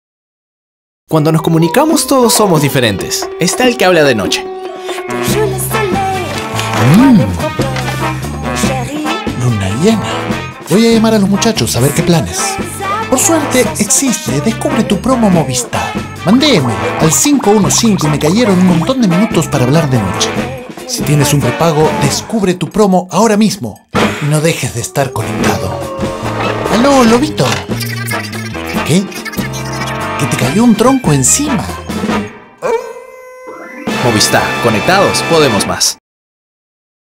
Movistar LOBO.mov
Segundo spot creado por Publicis para Descubre tu Promo de Movistar.

Ficha técnica
Cliente: Telefónica Móviles
Producto: Movistar
Agencia: Publicis Perú
Campaña: Descubre tu promo
Motivos: Paloma y Lobo
DCG: César de María
Director creativo: Marco Llontop
Redactor: Marco Llontop
Director de arte: Alberto Matsumoto, Gustavo San Cristóbal
Directora de Cuentas: Loreta Giribaldi
Sup. de Cuentas: Claudia Sotomarino
Producción Agencia: Raquel Becerra
Casa Realizadora: 7Samurai
Director: Jorge Caterbona
Director de fotografía: Fergan Chávez
Director de arte: Sandro Angobaldo
Producción: Por 2
Post Producción: Zeppelin
Audio: La Sound Facktory
Aprobación por el cliente: Luis Eduardo Garván / Catherine Sevillano de Smedt / Gisella Márquez